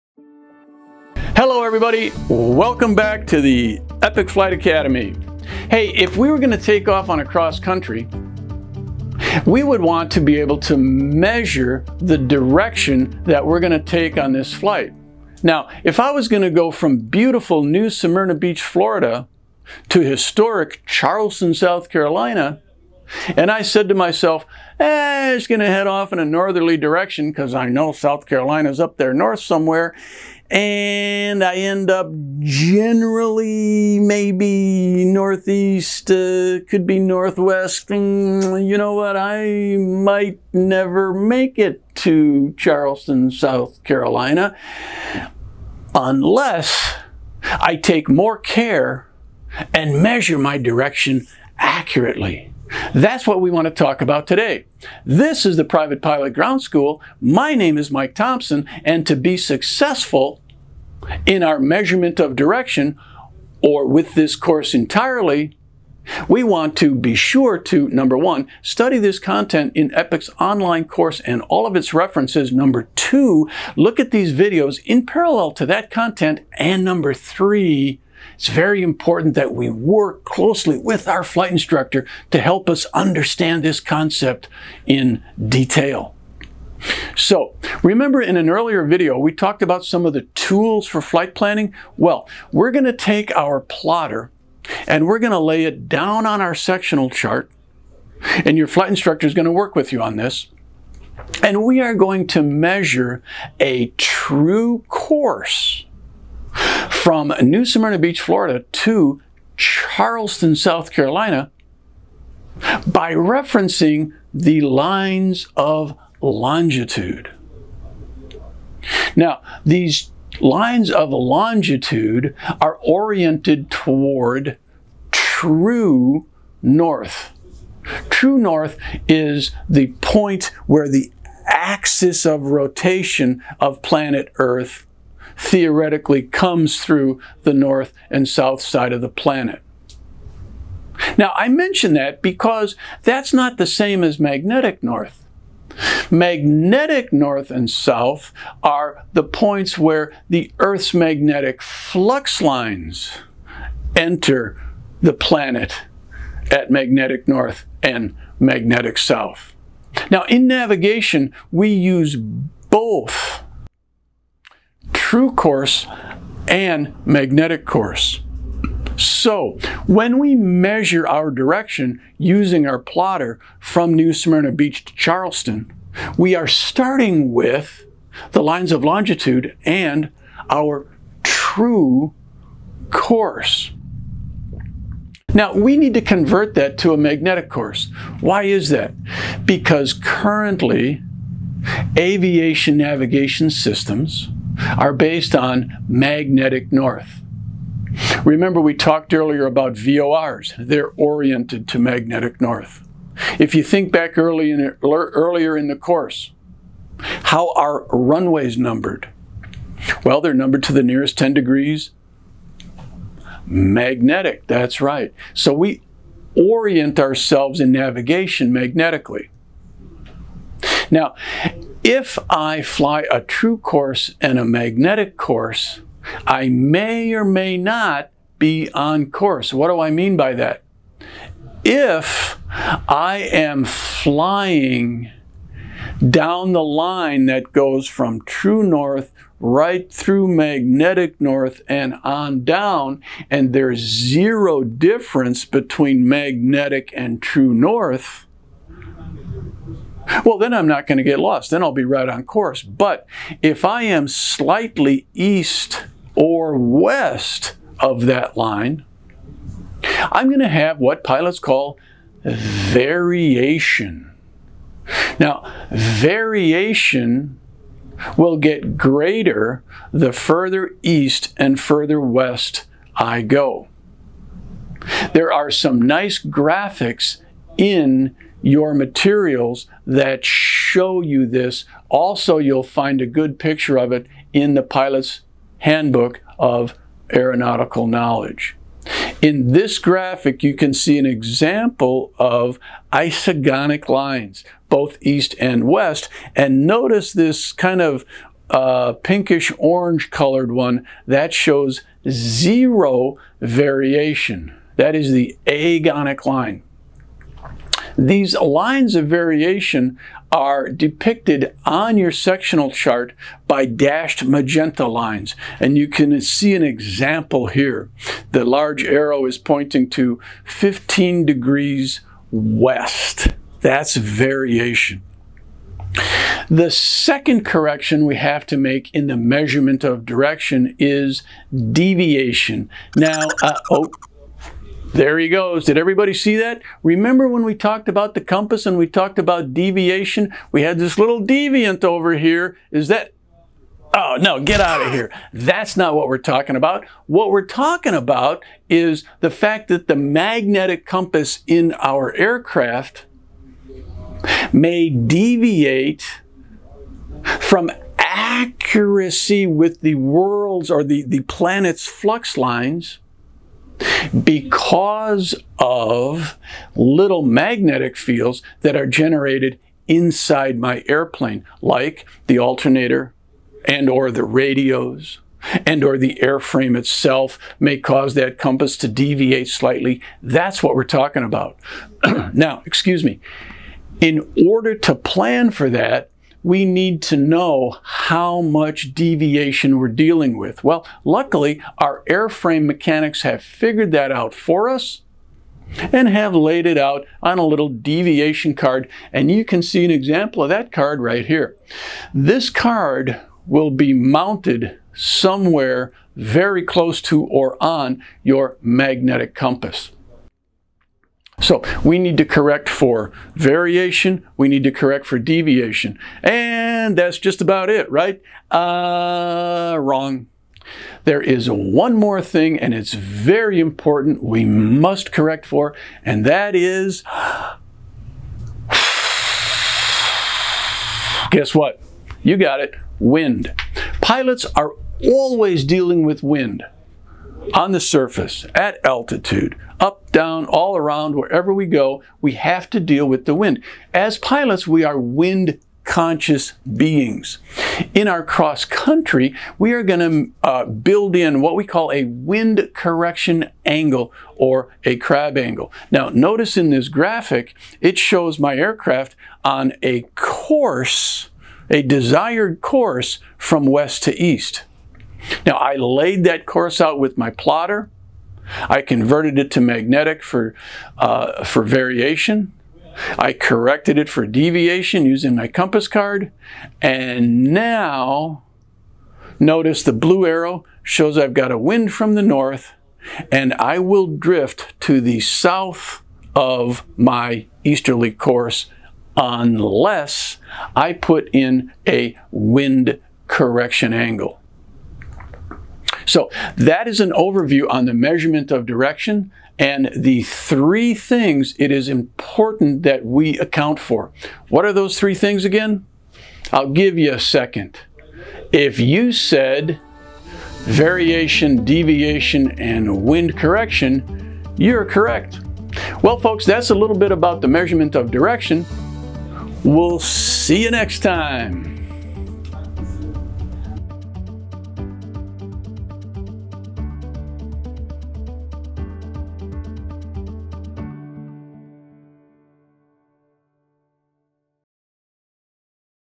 PPGS Lesson 12.5 | Navigation: Measurement of Direction
Welcome back to Epic Flight Academy’s Private Pilot Ground School!
In this Navigation lesson, Certified Flight Instructor Mike Thompson explains how pilots measure and correct for direction during flight.

You’ll learn how to determine true course, convert between true north and magnetic north, and understand how variation, deviation, and wind correction angles affect your heading and navigation accuracy. Mike also demonstrates how to read an isogonic line, interpret a deviation card, and apply these corrections during cross-country flight planning.

By the end of this lesson, you’ll be able to measure, plot, and adjust for all directional factors that influence real-world flight navigation.

📘 Video Chapters

0:00 – Introduction
1:42 – Measuring a True Course with the Plotter
2:08 – What Is True North?
2:26 – What Is Magnetic North?
3:05 – Why Do We Need to Convert to a Magnetic Course?
4:25 – What Is Variation?
4:52 – Isogonic Lines
5:33 – What Is Deviation?
6:42 – Deviation Card
7:14 – How Do We Correct for Wind? (Wind Correction Angle)
7:39 – Diagram Showing a Wind Correction Angle
8:18 – Review